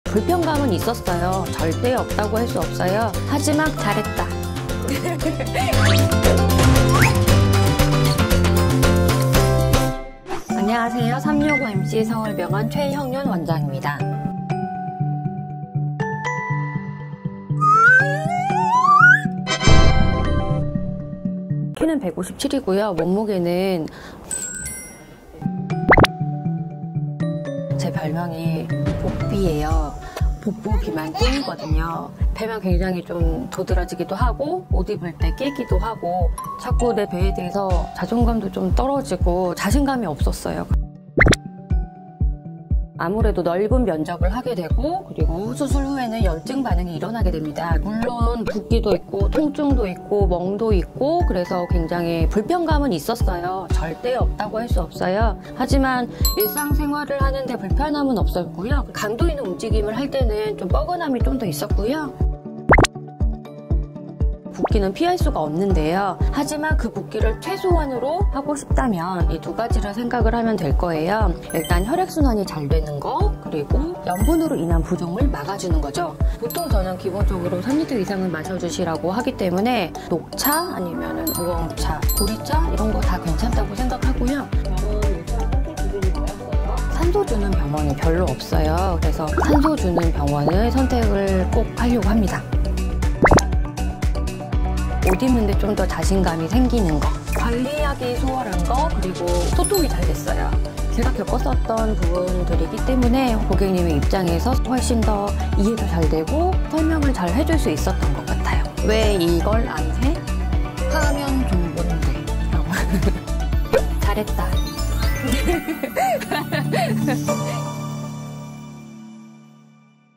내돈내받💸 | 지흡닥터👩‍⚕️가 직접 지방흡입 받아본 썰 (feat. 지흡효과, 관리꿀팁, 병원선택)
지방 뽑아내기만 하다가 직접 뽑혀본 지흡닥터의 찐 경험담!
전문가의 시각에서 나오는 진정한 수술후기와
놓칠 수 없는 리얼 꿀팁 가져가세요~